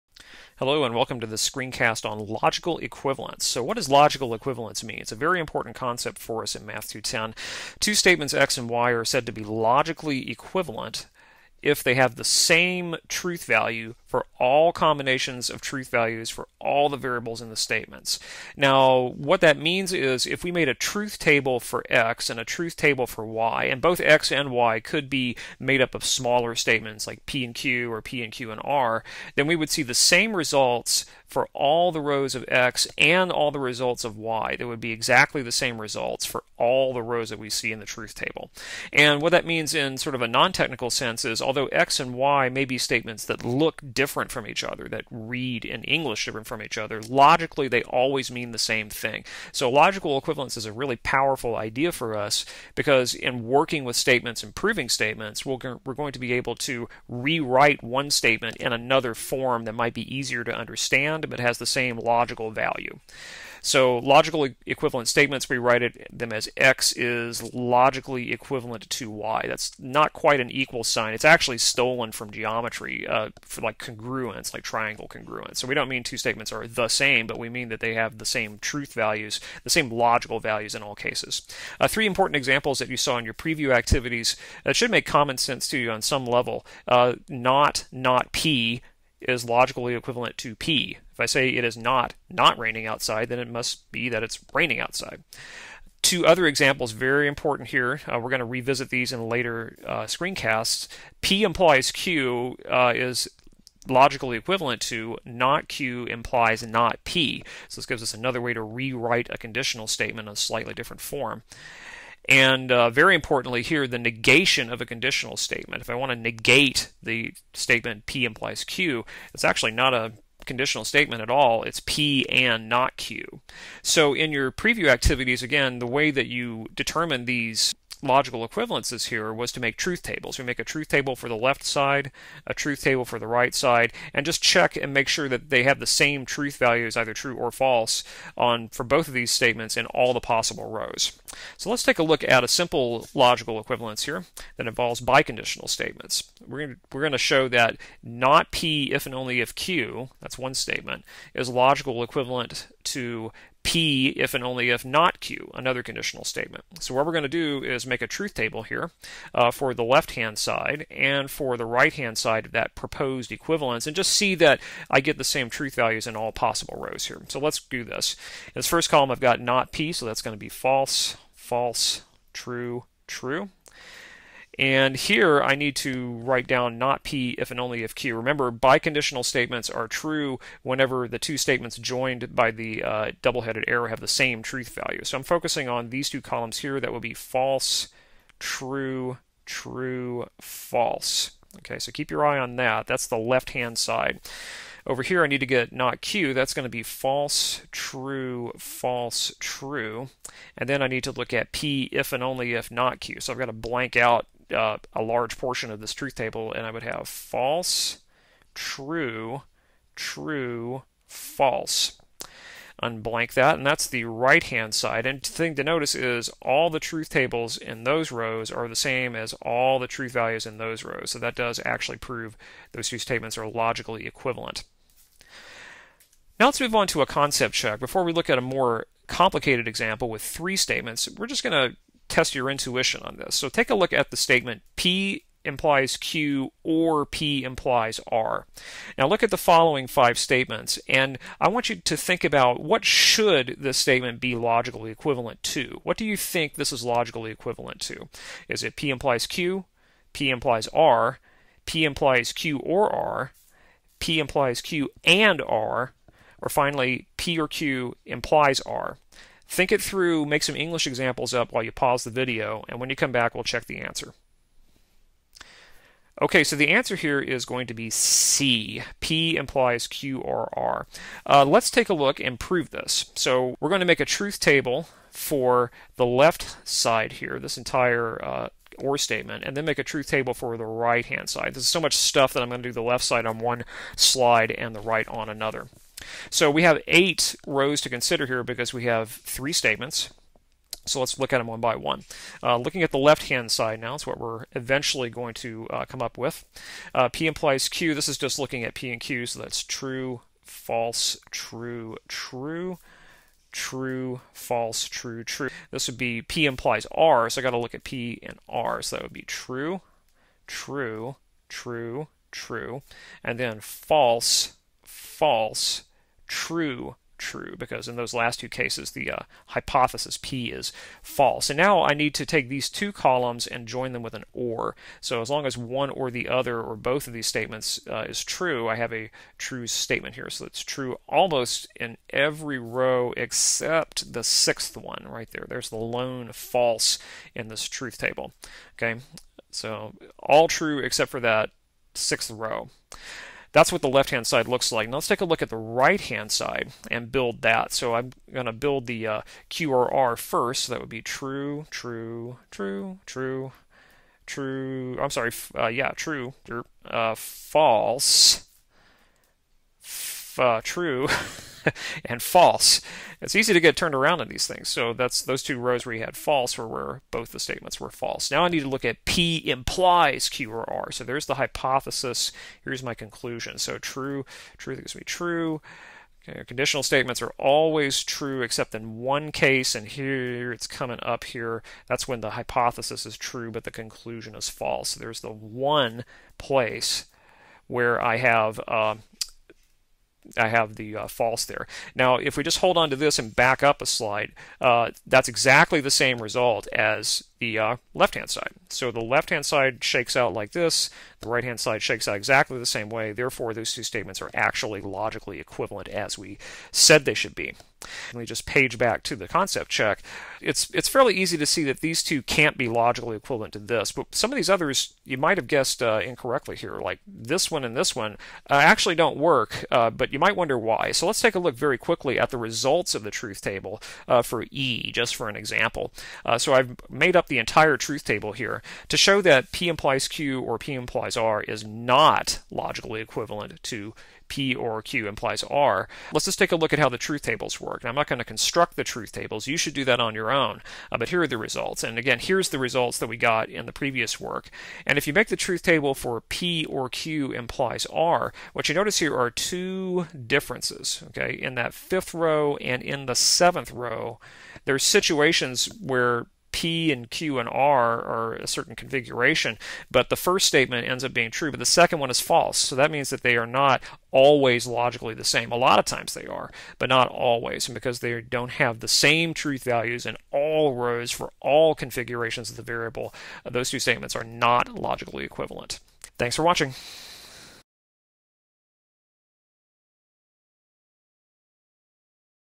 Logical equivalence (Screencast 2.2.1)
This video defines the notion of logical equivalence of statements. We show two examples of using truth tables to establish logical equivalence of statements and look at what it means for two statements NOT to be logically equivalent.